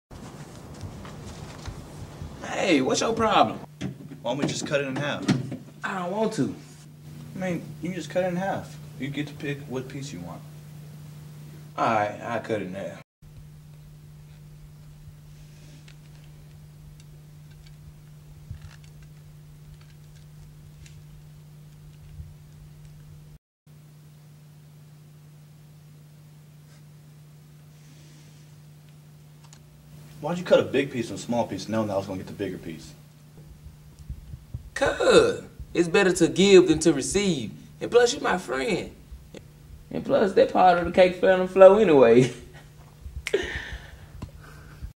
Shepherd Film Academy- Position One Camera Shots
Shepherd Film Academy Student project.  Resolve a quick story line shooting three Position One shots: Over the Shoulder, Face to face, Low Angle Reverse shot.  For more information about check us on on the web